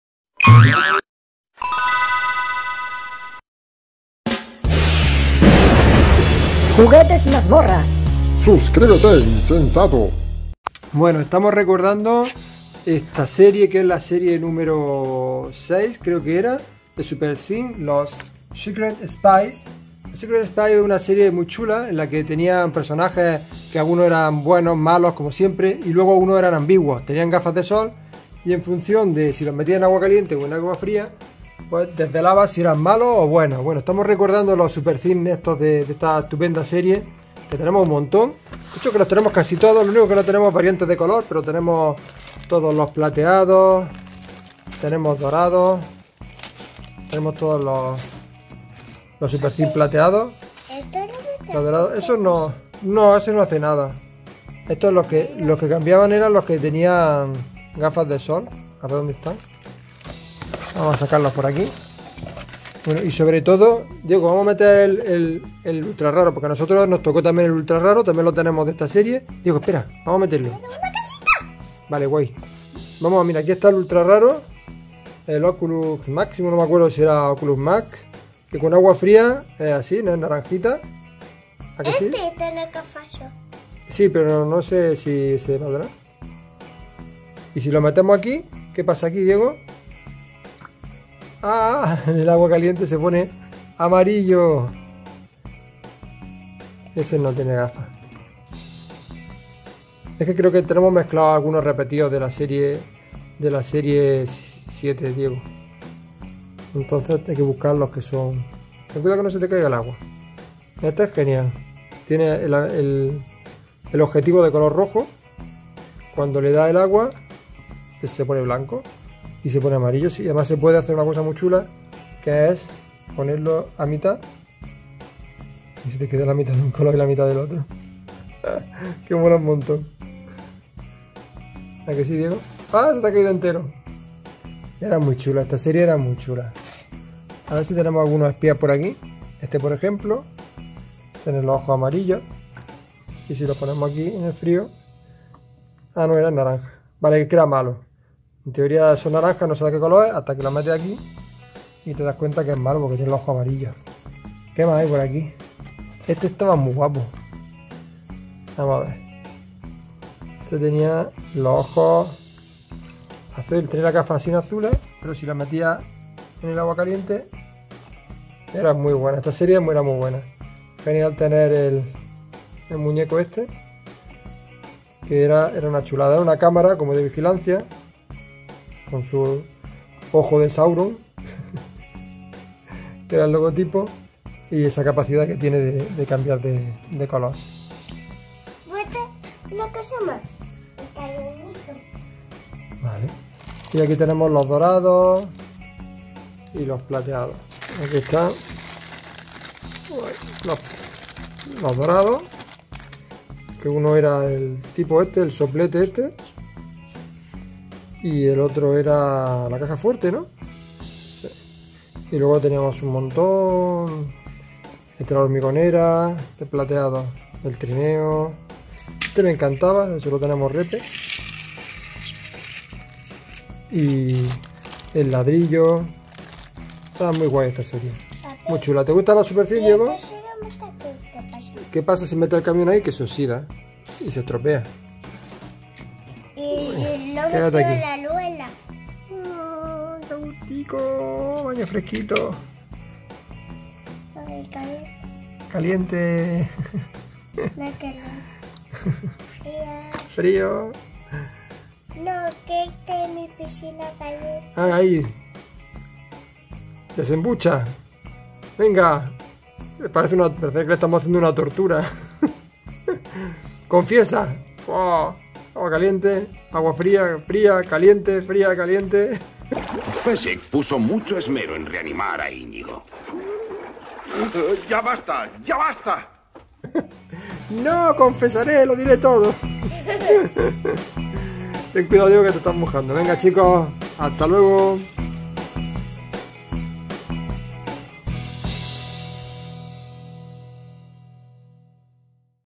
OCULUS MAX Ultra raro de la serie 6 de los Super Things "Secret Spies"
Vemos por encima la seri 6 de los super things, secret spies, y vemos como funciona el poder del Ultra raro de la serie, OCULUS MAX.